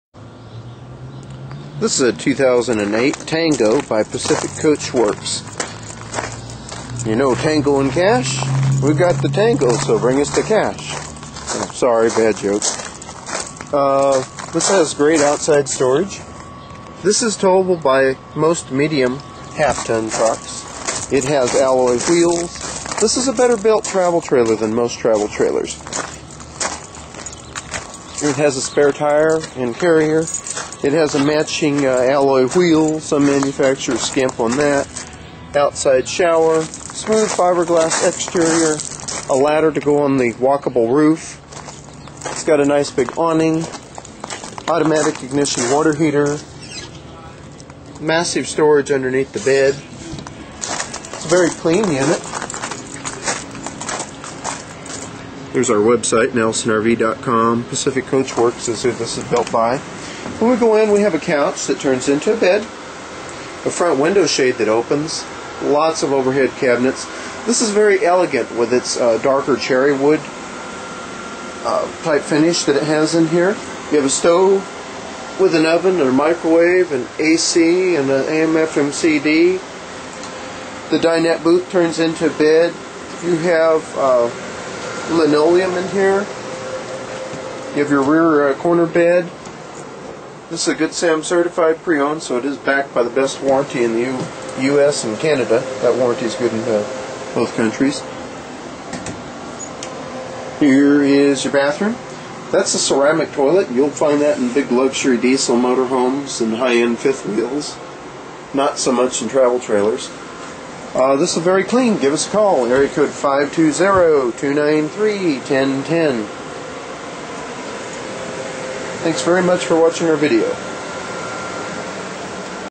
2008 Tango 216CB TT for sale @ Nelson RV in Tucson AZ
This 2008 Tango has 2 door refrigerator, AC, 6 gal water heater, ceramic RV toilet, and other features.  You can see this nice clean at Nelson RV in Tucson AZ,